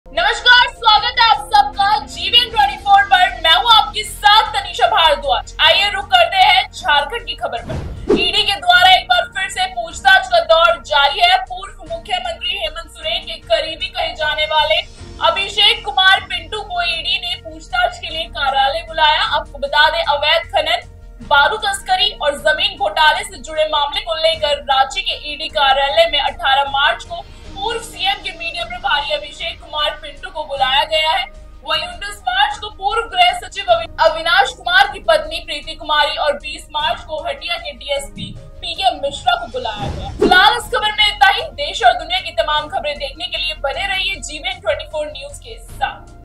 Jharkhand: Abhishek Kumar को ईडी ने पूछताछ के लिए कार्यालय बुलाया | Gbn24
ईडी के द्वारा एक बार फिर से पूछताछ का दौर जारी है...पूर्व मुख्यमंत्री हेमंत सोरेन के करीबी कहे जाने वाले अभिषेक कुमार पिंटू को ईडी ने पूछताछ के लिए कार्यालय बुलाया

Gbn24 News | Gbn24 | Hindi News | Gbn24 News Live